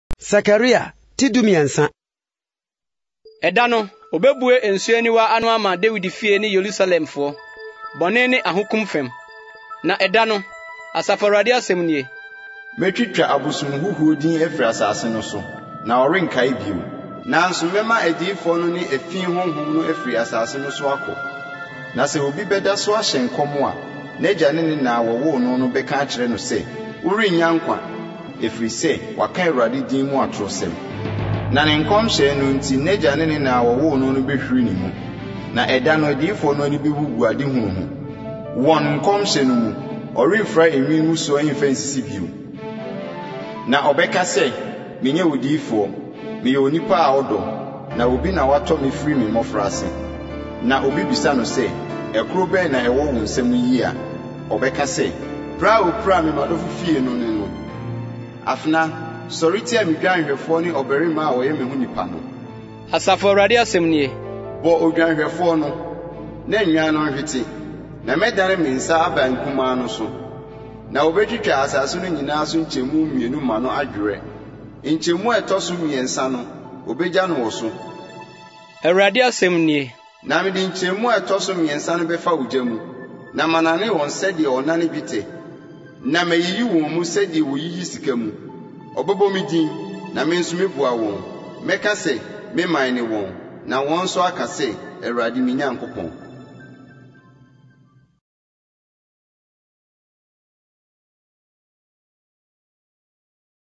✝️Zechariah Chapter 13 - Audio Twi Bible reading 📖
0:00 Intro
0:16 Reading
0:21 Reading of verses
( Romans 10:17)

Nhyira Nka Asafo Awurade:
Amen !!! 🙏🏼🙏🏼